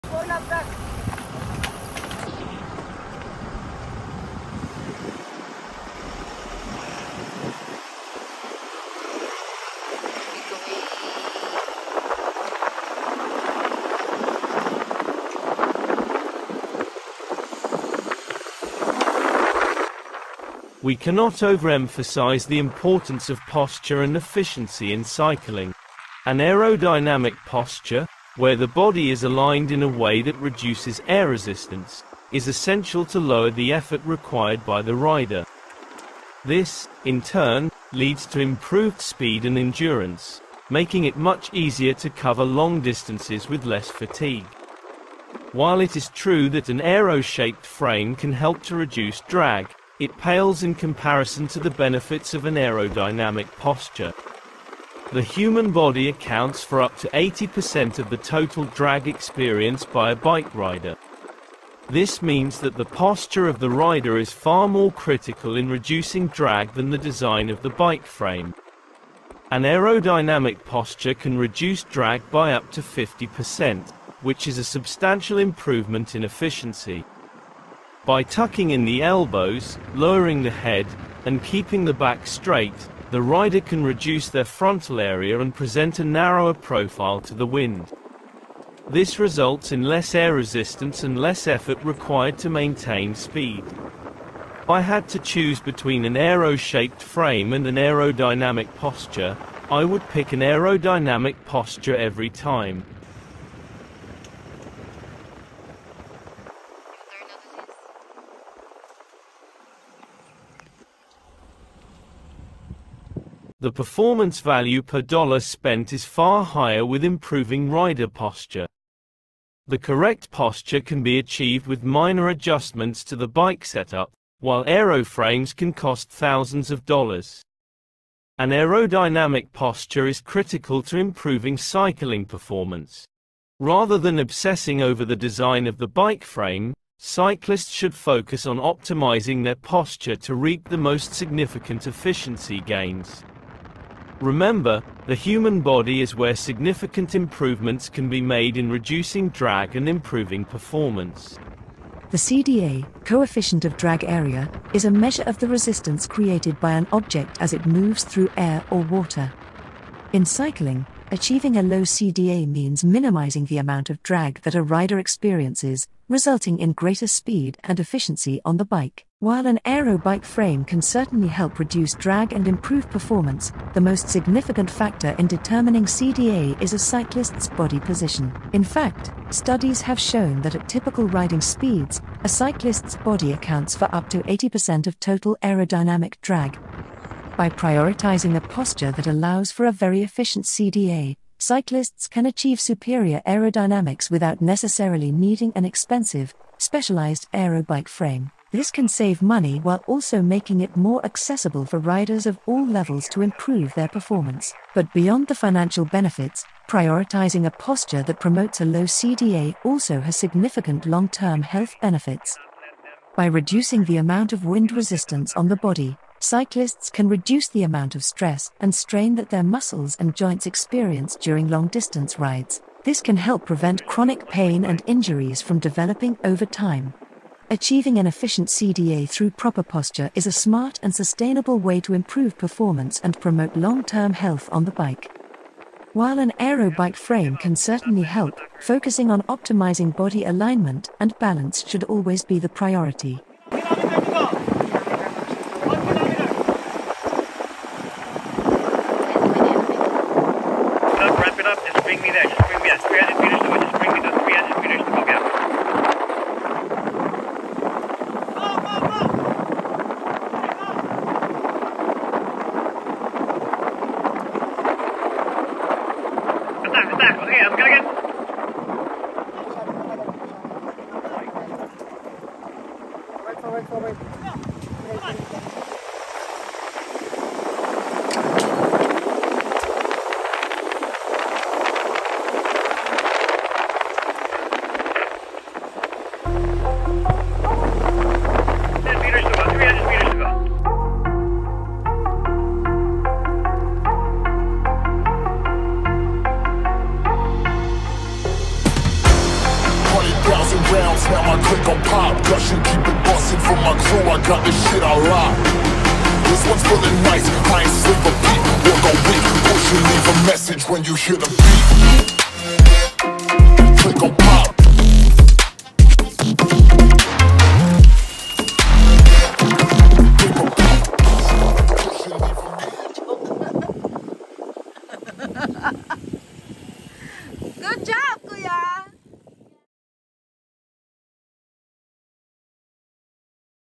Benefits of Low CdA Posture
Low CdA also has significant long-term health benefits. By reducing the amount of wind resistance on the body, cyclists can reduce the amount of stress and strain that their muscles and joints experience to go faster during long-distance efforts. This can help prevent chronic pain and injuries from developing over time & has an indirect effect of lowering heart rate